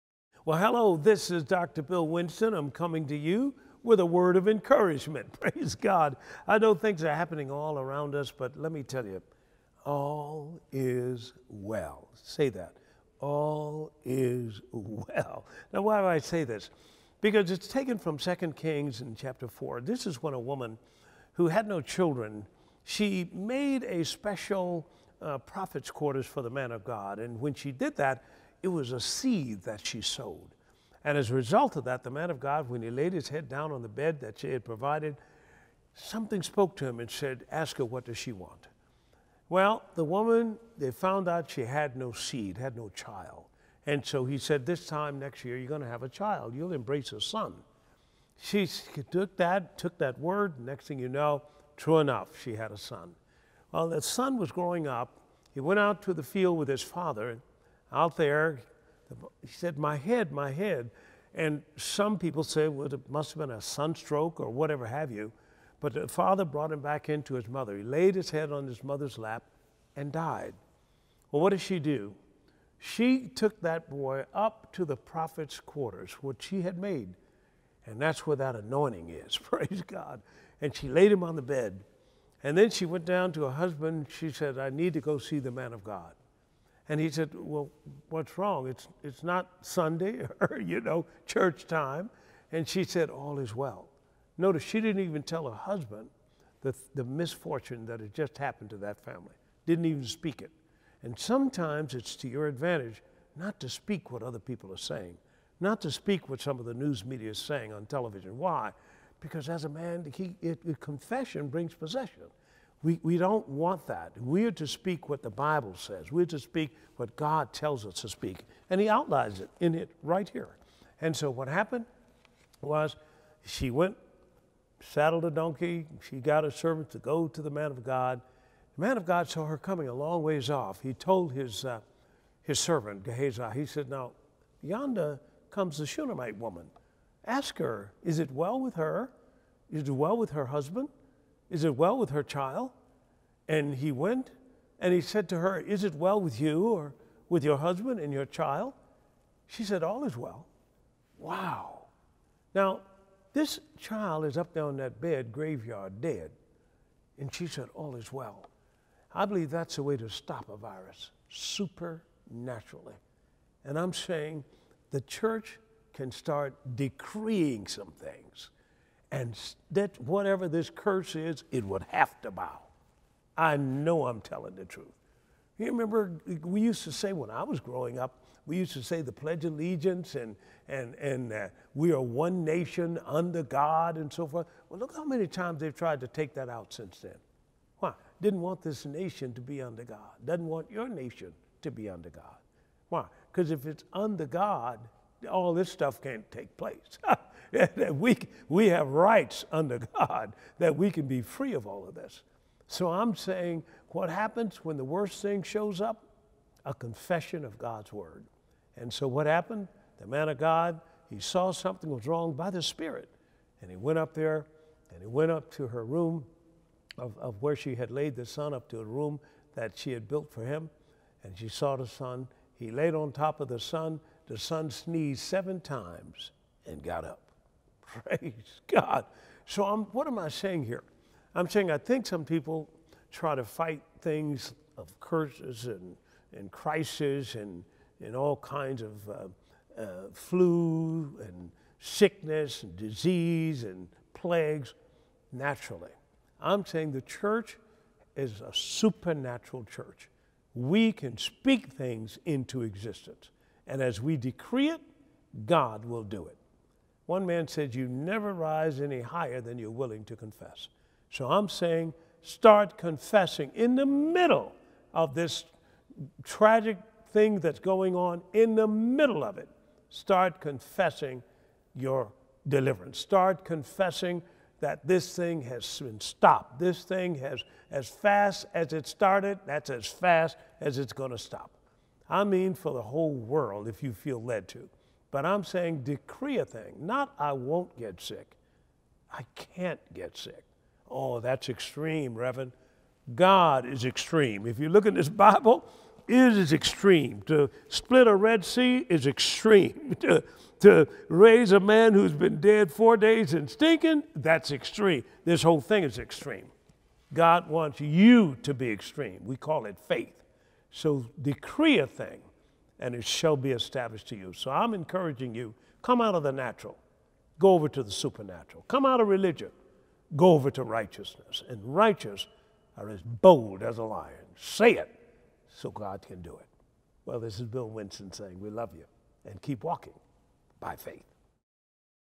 All is Well
Dr. Winston shares on the power of our confession! Take a listen and be encouraged.